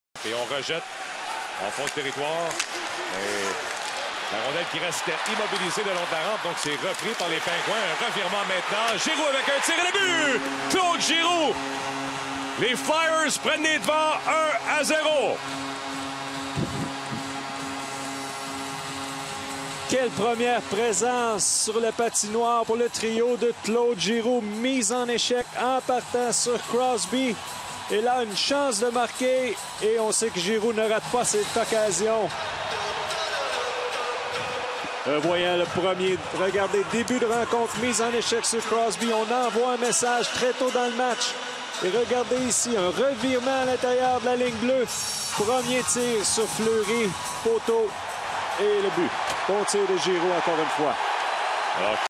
Claude Giroux destroys Sidney Crosby and scores a nice goal on Marc-Andre Fleury [HD]
Giroux begins the game strongly by hitting hard Sidney Crosby and by scoring a beautiful goal on Marc-Andre Fleury